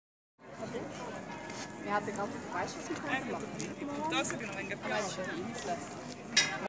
Vista su "the Floating Piers"
Veduta dal sentiero della Valeriana in direzione Sulzano con vista sull'evento the Floatingpiers